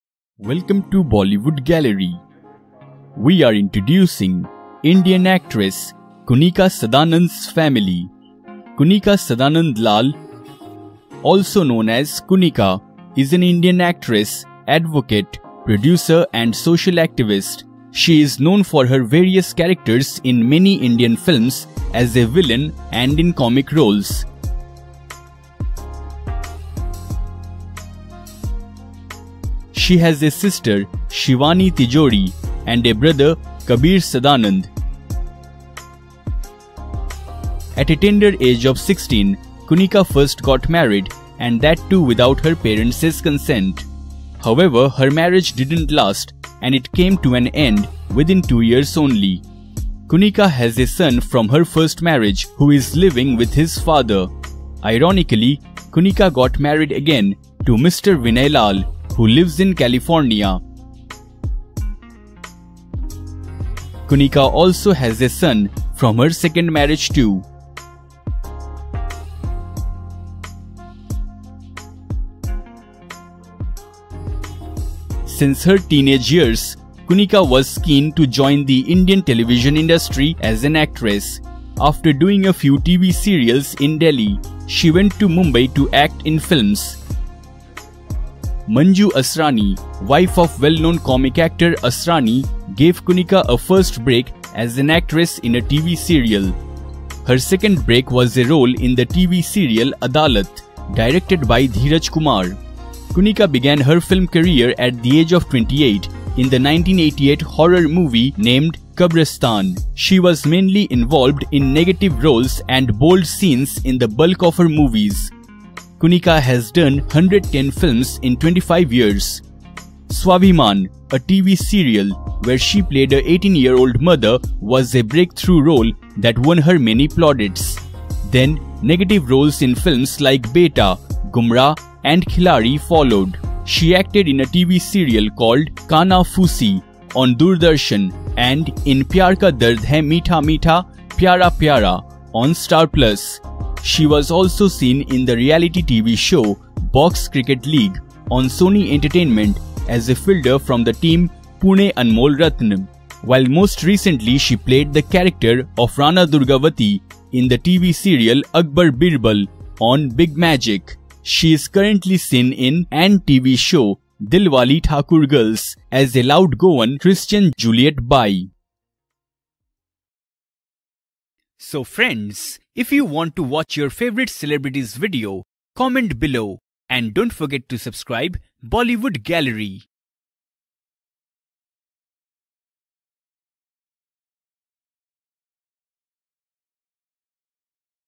Kunickaa Sadanand Lal Family With Parents, Husband, Son, Brother, Sister & Biography
Kunickaa Sadanand Lal, also known as Kunika, is an Indian Actress, advocate, producer and social activist. She is known for her various characters in many Indian films as a villain and in comic roles.

She has a Sister Shivani Tijori and a brother Kabir Sadanand.

At a tender age of sixteen, Kunika first got married and that too without her parent's consent. However, her marriage didn't last and it came to an end within two years only.

Kunika has a son from her first marriage who is living with his father.

Ironically, Kunika got married again to Mr Vinay Lall who lives in California. Kunika also has a son from her second marriage too.